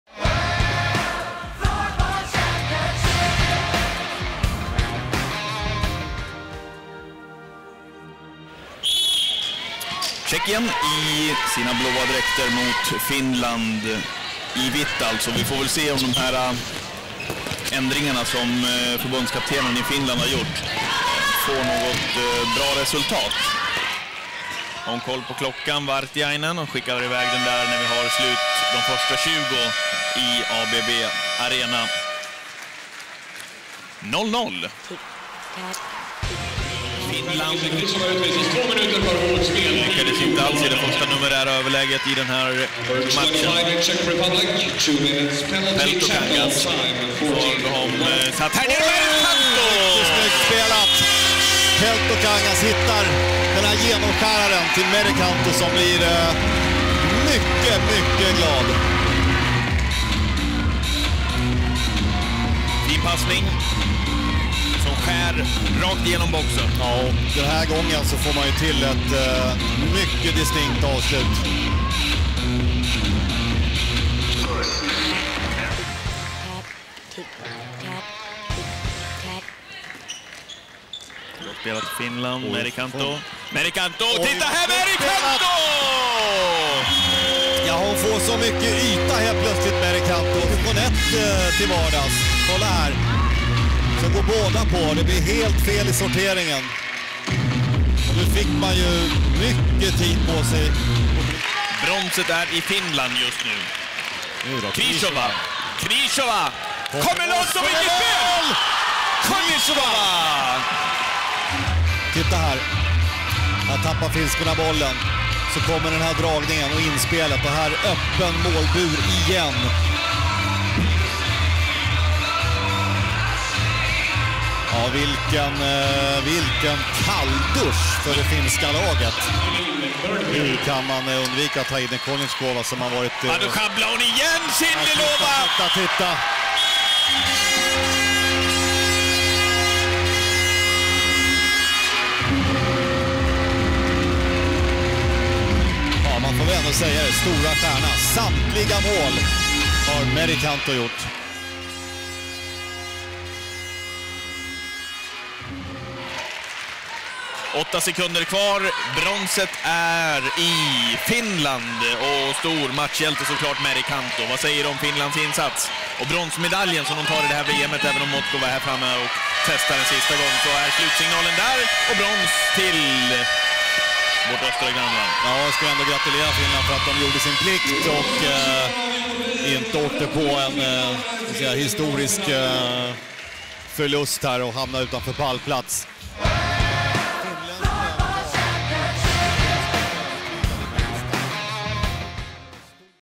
CZE-FIN Women's WFC 2009 Bronze match Highlights
Highlights from women's World Floorball Championships bronze match Czech Republic - Finland (1-3)